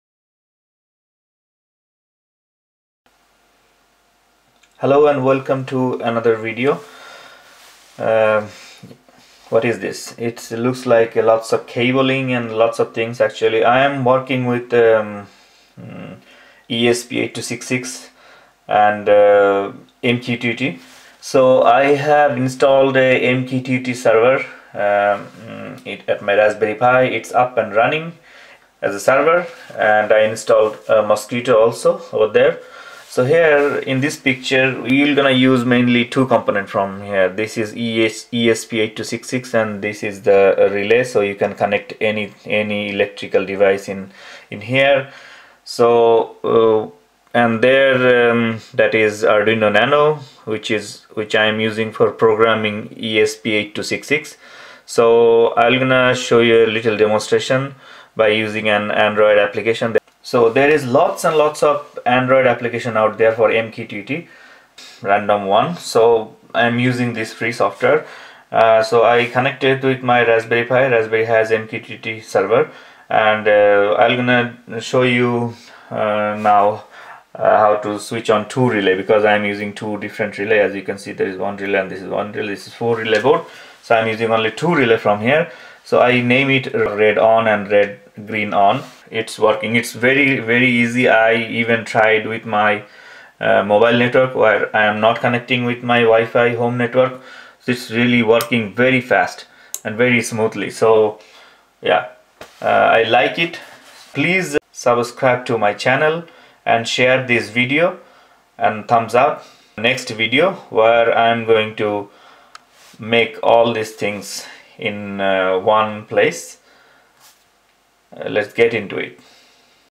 ESP8266 MQTT
I am using RaspberryPi as MQTT server and ESP8266 (esp-01) which is connected with realy board. There is lots of free Android MQTT software which is easy to use. I will add more video for complete project. It's working super fast even I am using mobile network or outside home network.